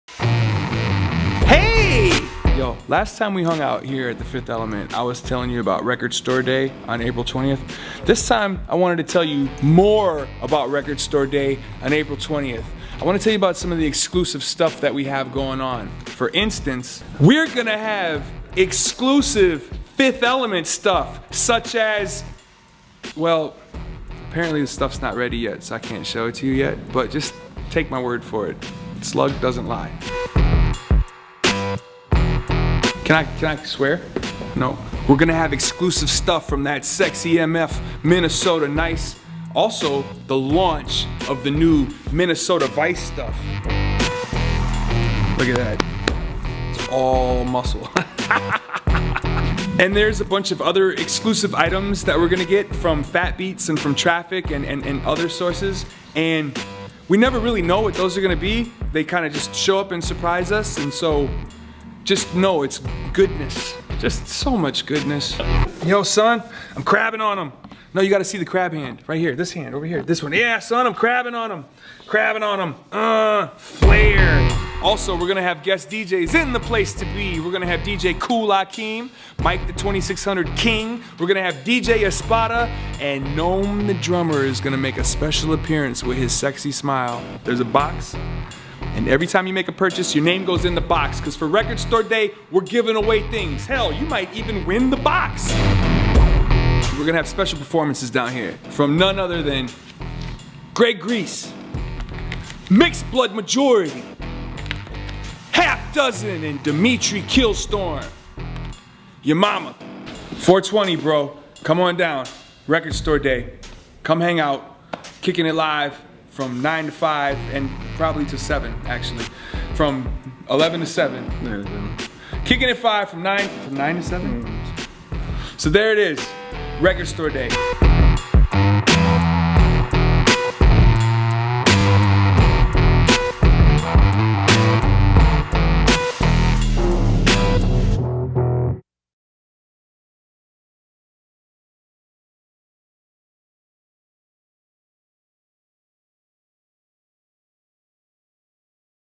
Fifth Element Record Story Day 2013: Ambassador Slug Part Two
for full RSD Details!!

Record Store Day at Fifth Element is on 4-20-13 and we are open from 9 AM-7 PM!!
There's a lot of excitement in the air around Record Store Day 2013! The FE and Rhymesayers staff is excited to unleash some new cool stuff. People, like yourself, have given some excited feedback and our ambassador Slug? Well, he's very excited to talk about all of it. See for yourself in part two, where he discusses FE Exclusives, the MN Vice T Shirt launch, our guest performers and DJs, all day giveaways, and so on...  All new T Shirts available In-Store and Online on 4-20-13 at 9 AM. Please note that RSD Exclusive Vinyl Items are not available online on Record Store Day during store hours (9 AM-7 PM). Follow the Fifth Element Facebook & Twitter at 9 PM CST 4-20-13 for any updated info!!

Directed by- Andrew Melby
Director of Photography- Jake Gardner